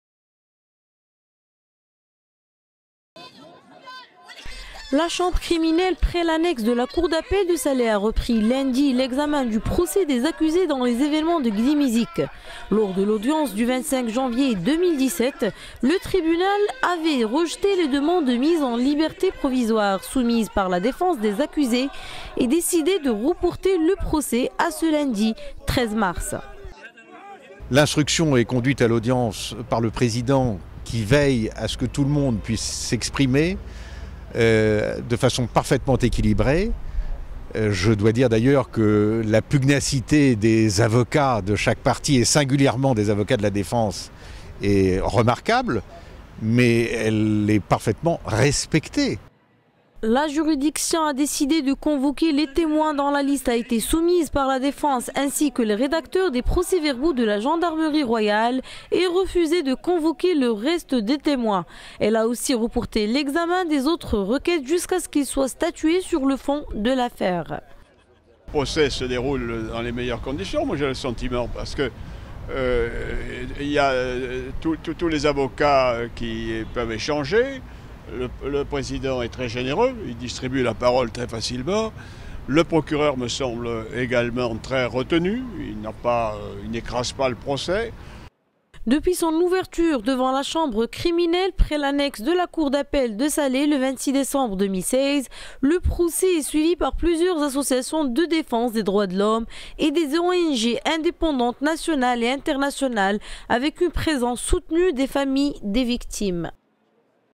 Reprise à Salé du procès des événements de Gdim Izik
La Chambre criminelle près l'annexe de la Cour d'appel de Salé a repris, lundi, l'examen du procès des accusés dans les événements de Gdim Izik, survenus en 2010 et ayant fait 11 morts et 70 blessés parmi les forces de l’ordre et quatre parmi les civils.